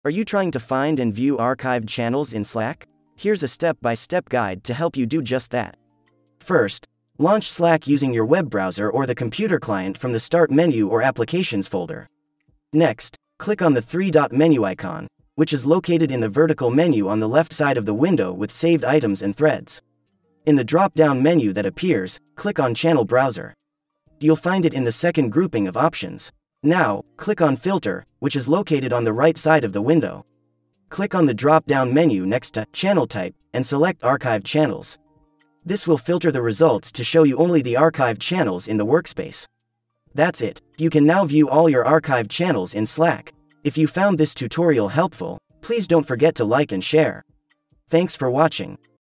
How to View Archived Channel in Slack (2025)
How to View Archived Channel in Slack.

Learn how to view archived channels in slack. It is really easy to do and learn to do it in just a few minutes by following this super helpful tutorial to get a better understanding of everything.